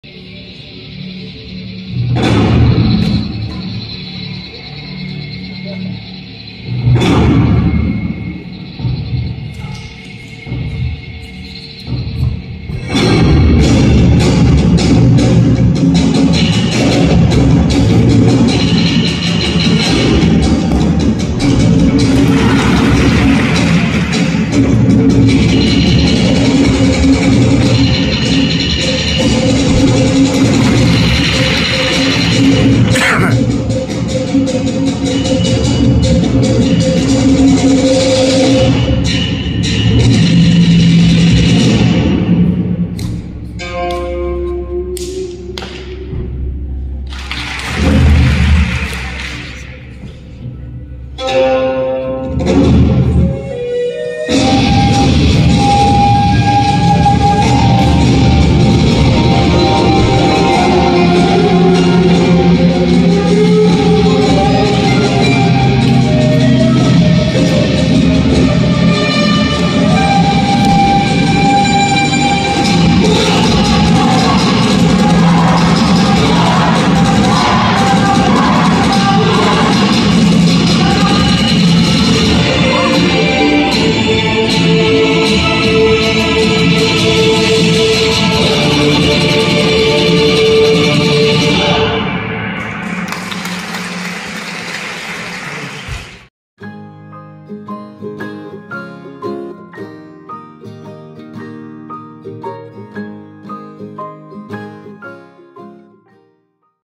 Explosive Changquan - testing in Gansu for the next China Taolu Nationals
You need to watch this unique explosive changquan (longfist) form! Lots of sharp traditional movements integrated with explosive nandu. Held as part of the mock competition as they prep in Gansu for hosting the next Wushu Taolu Nationals in China.